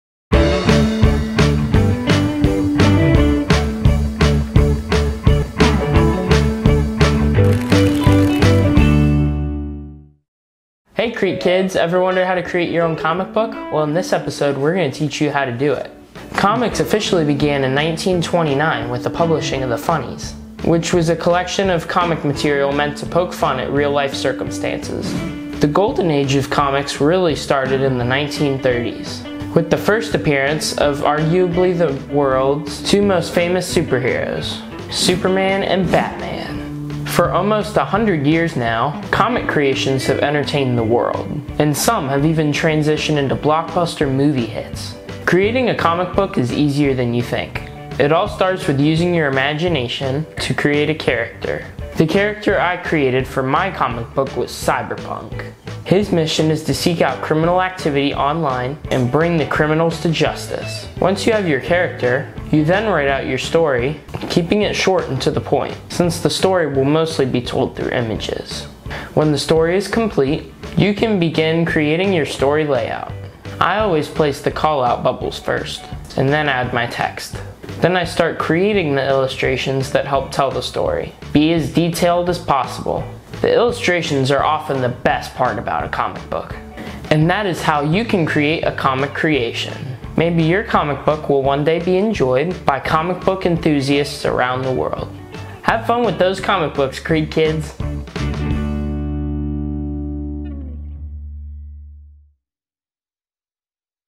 CreekTALK Kids Making a Comic Book
A fun activity for kids on those rainy summer days in Coconut Creek.

CreekTALK Kids
A CreekTV Production

City of Coconut Creek, Florida
CoconutCreek.net